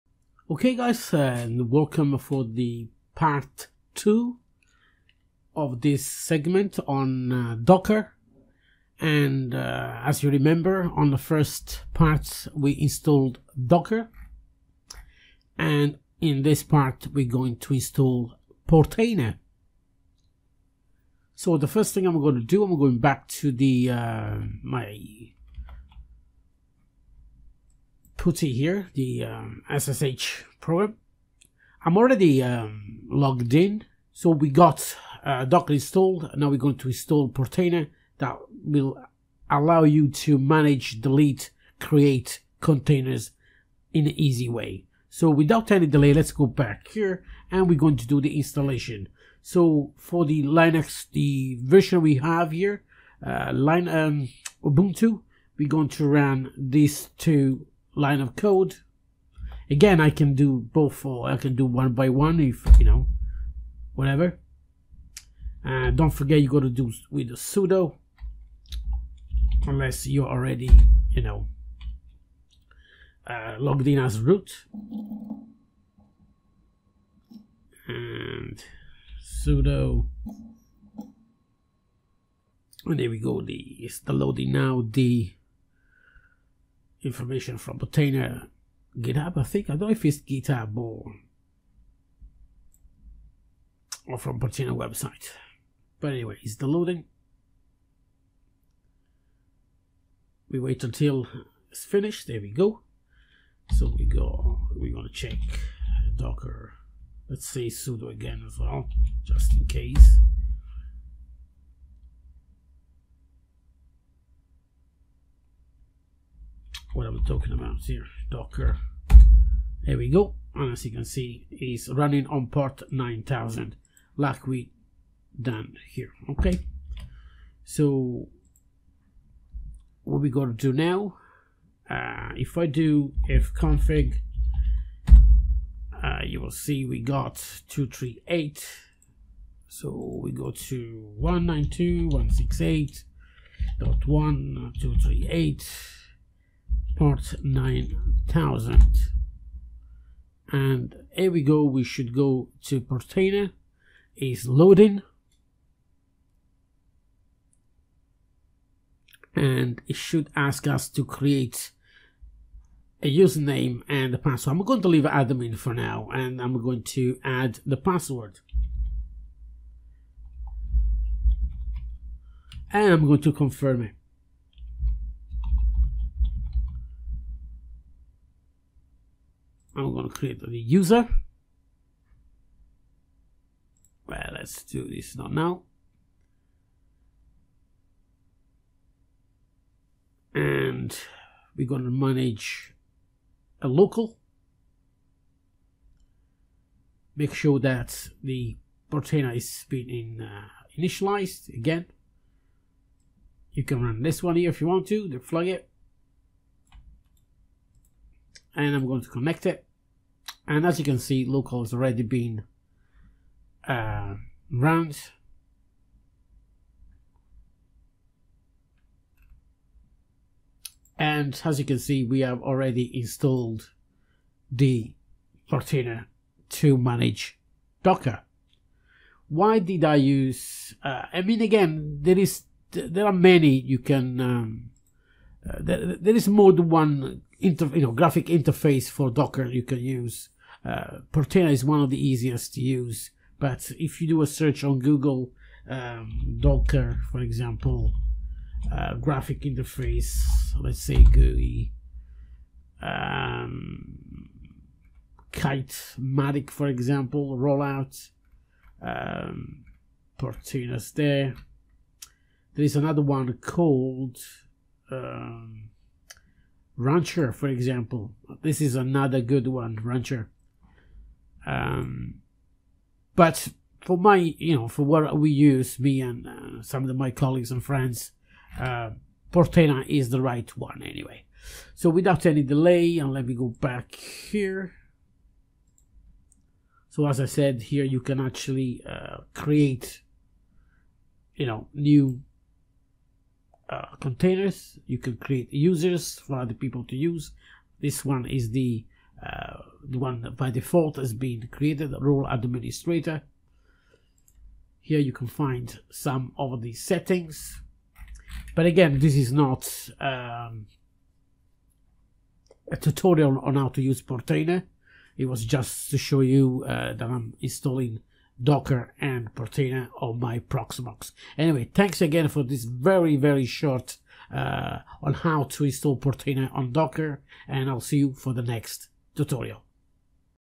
How to install Portainer GUI on Docker to manage it easily - Part 2/3
Portainer is a lightweight management UI which allows you to easily manage your Docker host or Swarm cluster. Portainer is meant to be as simple to deploy as it is to use. It consists of a single container that can run on any Docker engine (Docker for Linux and Docker for Windows are supported).